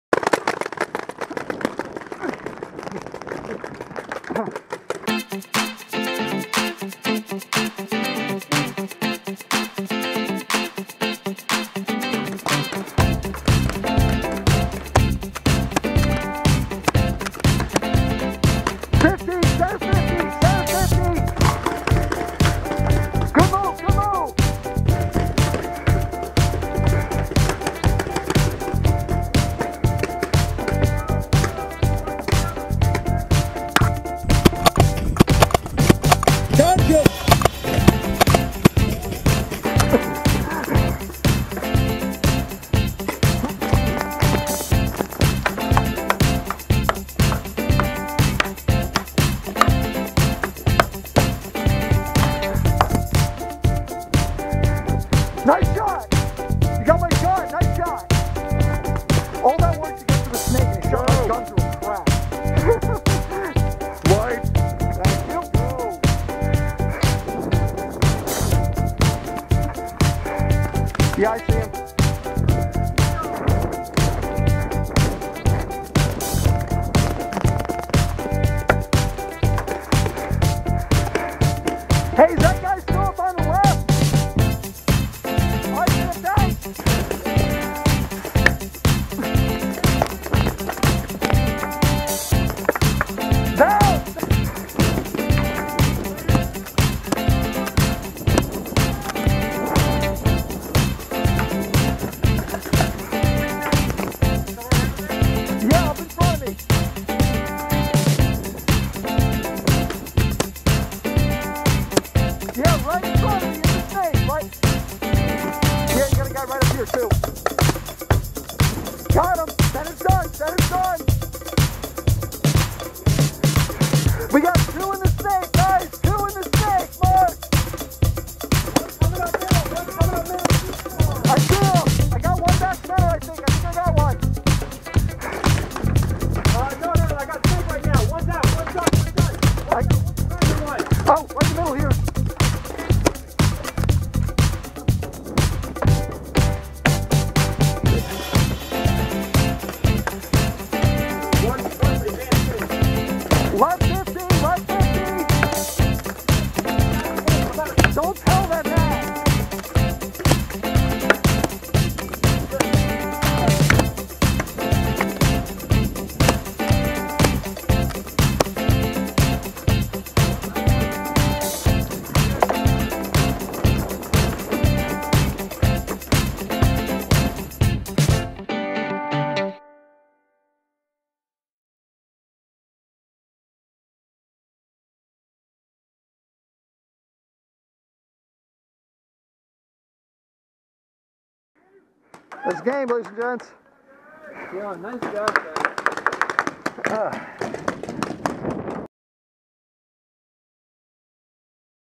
Darwin FPS 51.3 - Dr. Paintball part 3
Way back in 2011, I was invited by a friend in medical school to a game with a bunch of med students. The footage from that day became FPS 8:

They're all about the graduate, and invited me back when they went to play again.

Same field (Top Gun Paintball in Cream Ridge, NJ), tons of fun.

A note about this one: I'm trying out a dual camera setup for the first time. Contour on the mask, GoPro on the barrel. The GoPro is set to shoot in 1080 with a narrow field of view, so it's somewhat better at a distance than the Contour. The goal was to make shots easier to see.

Last game of the day, on the hyper pipes field. This was a respawn game; after you got hit, you could tag back in. If you got hit a second time, you were out for good.